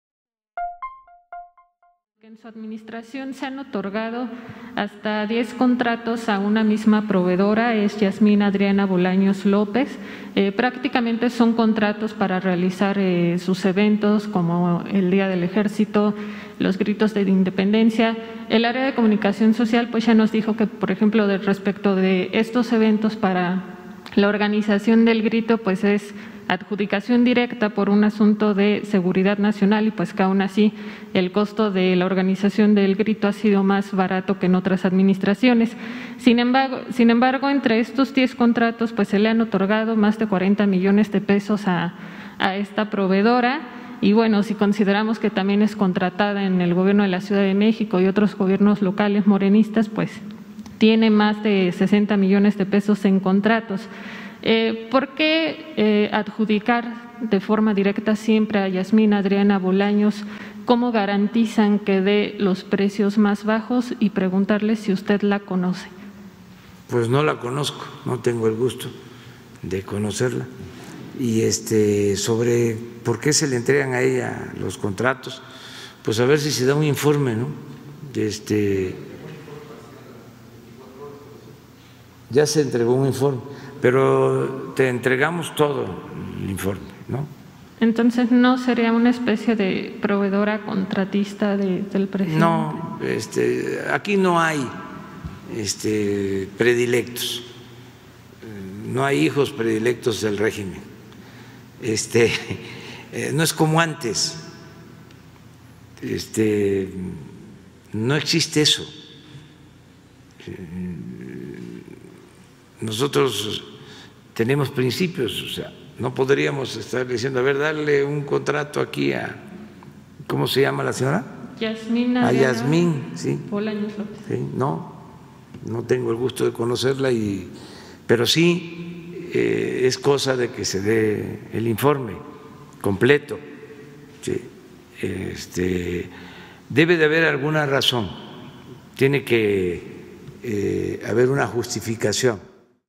En 4T 'no hay hijos predilectos'
El Mandatario federal pidió que se verificara el asunto de la adjudicación de contratos a ciertas empresas que han sido favorecidas por la Administración.